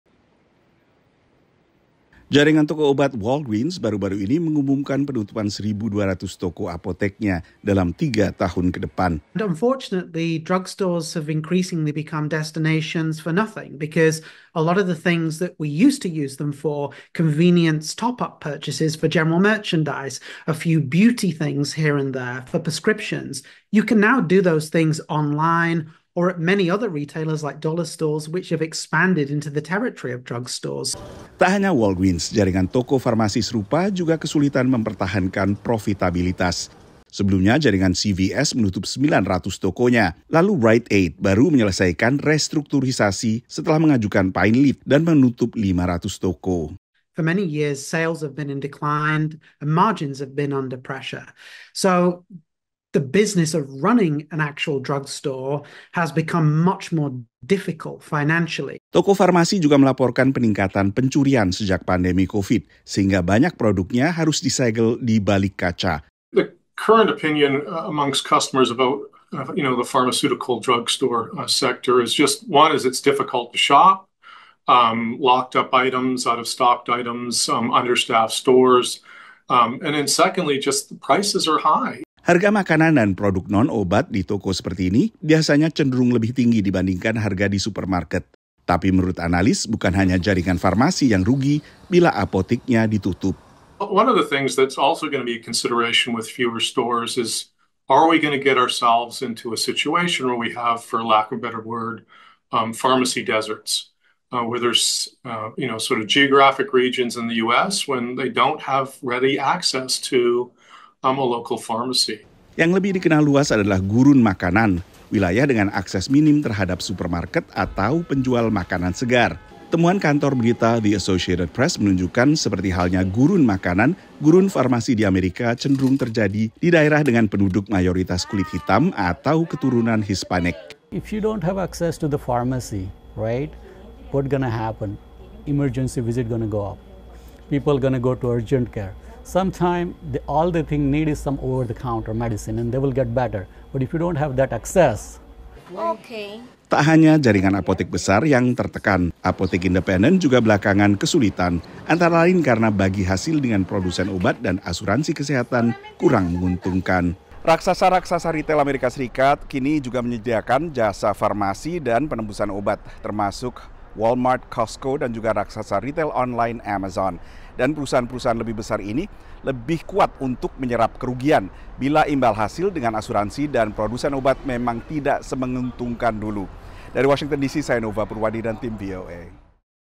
Penutupan Massal Toko Obat di Amerika dan Kekhawatiran Banyaknya "Gurun Farmasi"
Tingginya inflasi dan berbagai perkembangan lain memukul pemasukan toko obat, mulai apotek independen hingga toko jaringan farmasi besar. Perkembangan ini memaksa penutupan ratusan toko, yang dikhawatirkan memutus akses banyak komunitas kurang terlayani terhadap obat resep dan tanpa resep dokter.